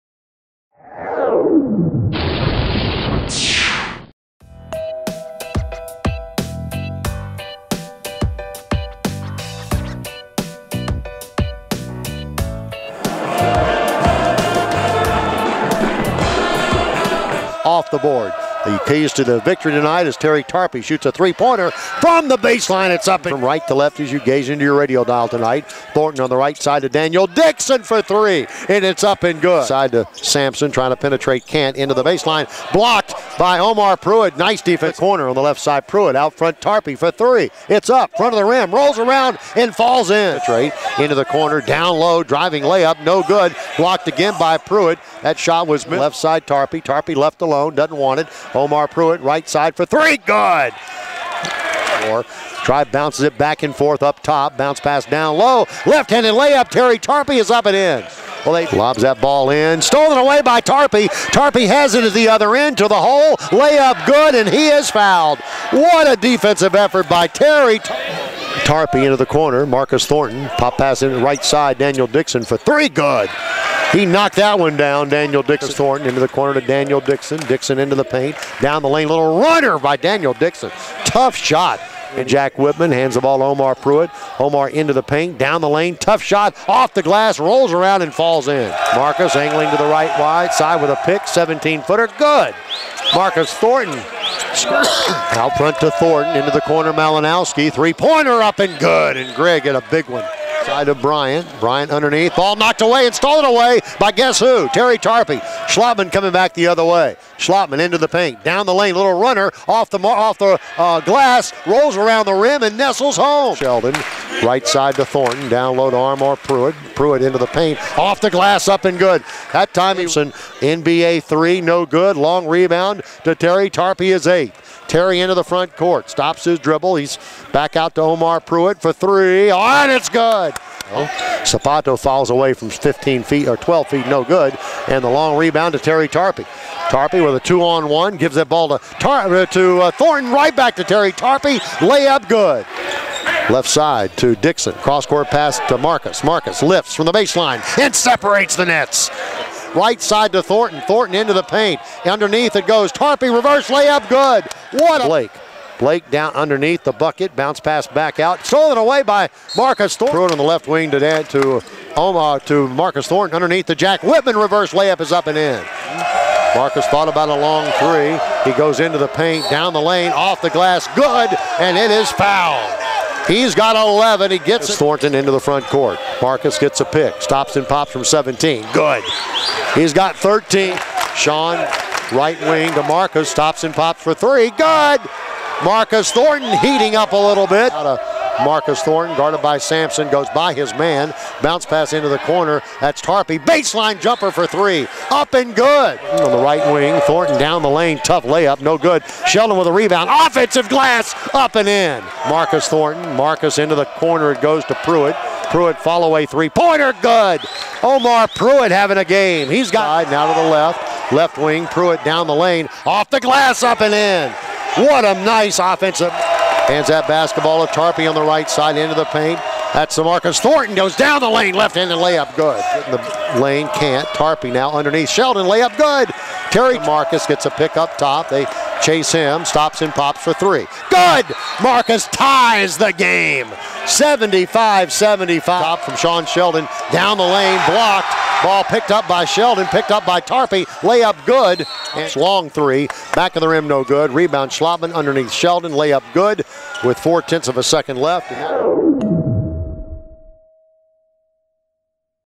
2014-15 William and Mary Men's Basketball Highlights at Elon
Check out the highlights from Tribe men's basketball's first CAA loss of the season at Elon, 85-79, on Jan. 8.